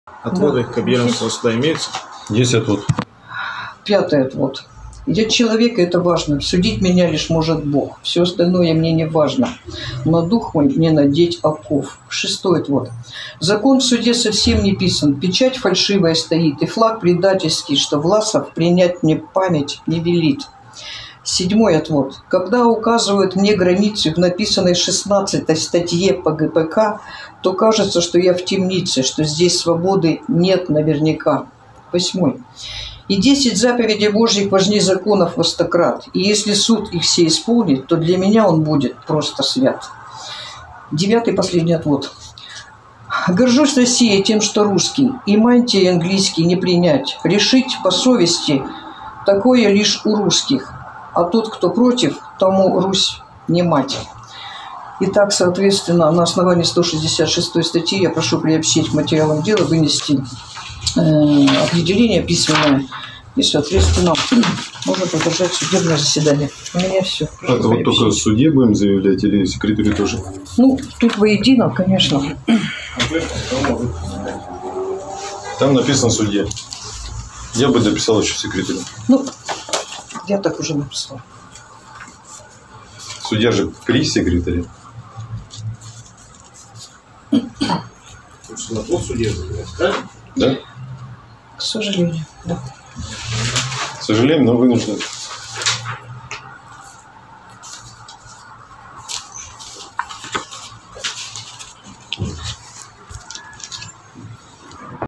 ОТВОДЫ СТИХАМИ или ТВОРЧЕСКИЙ ПОДХОД 2022.12.13 Сургут
учитывая что почти все отводы чёрные мантии отказываются удовлетворить решили применить стихотворную форму отводов

на ты или на вы?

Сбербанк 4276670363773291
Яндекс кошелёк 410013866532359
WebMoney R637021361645